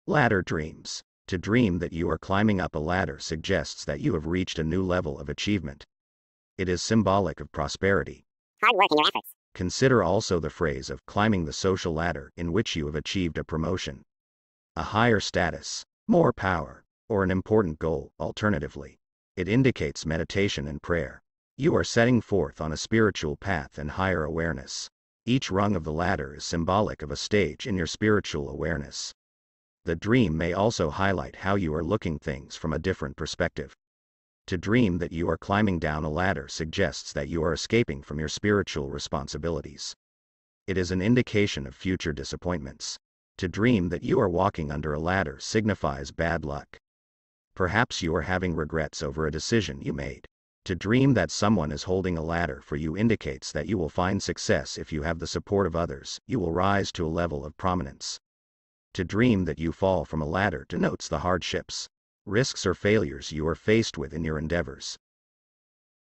Ladder Dreams​
To dream that you are climbing up a ladder suggests that you have reached a new level of achievement. It is symbolic of prosperity, hard work and your efforts. Consider also the phrase of "climbing the social ladder" in which you have achieved a promotion, a higher status, more power, or an important goal.  Alternatively, it indicates meditation and prayer. You are setting forth on  a spiritual path and higher awareness. Each rung of the ladder is symbolic of a stage in your spiritual awareness. The dream may also highlight how you are looking things from a different perspective.

To dream that you are climbing down a ladder suggests that you are escaping from your spiritual responsibilities. It is an indication of future disappointments.

To dream that you are walking under a ladder signifies bad luck. Perhaps you are having regrets over a decision you made.

To dream that someone is holding a ladder for you indicates that you will find success if you have the support of others. You will rise to a level of prominence.

To dream that you fall from a ladder denotes the hardships, risks or failures you are faced with in your endeavors.

To see a broken ladder in your dream indicates consistent failures in your undertakings. You may feel that you are being handicapped in pursuing your goals.

To dream that you escape by means of a ladder signifies that you will be successful after much struggle and obstacles.

See The Meaning In Action  Stuck At The Top

©BIG CREDIT​ PICTURE FROM GOOGLE, PINTEREST, etc.